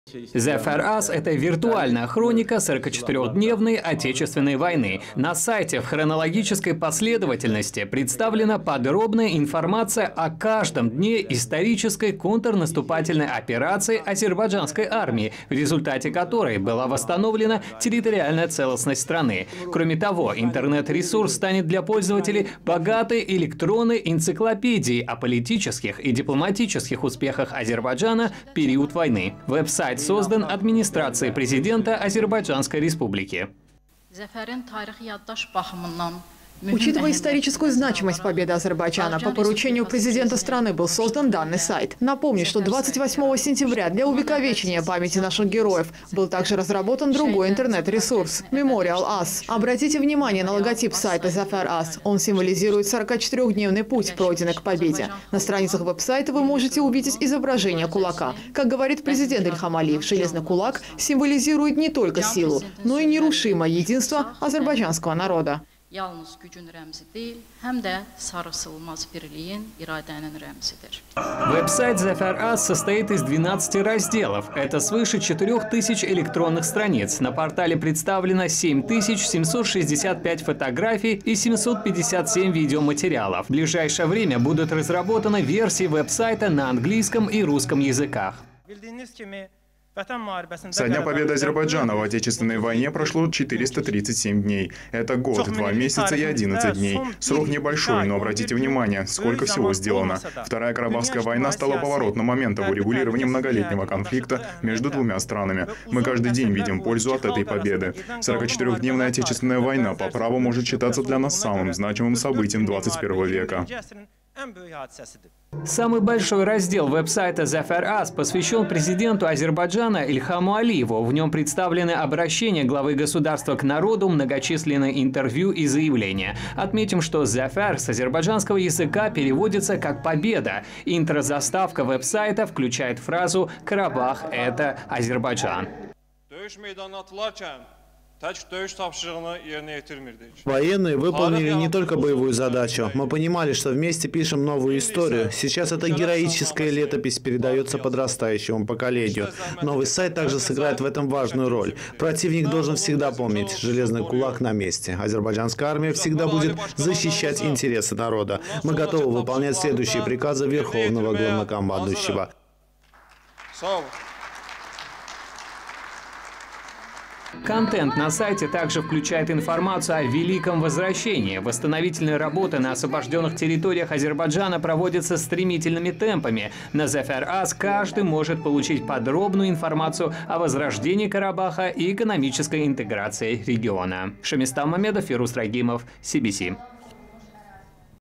В Баку презентован веб-сайт
В Баку в Центре Гейдара Алиева состоялась презентация веб-сайта «zefer.az». Интернет-ресурс станет «виртуальной летописью» победы Азербайджана во Второй Карабахской войне. Интернет-ресурс содержит тысячи фотографий и видеоматериалов.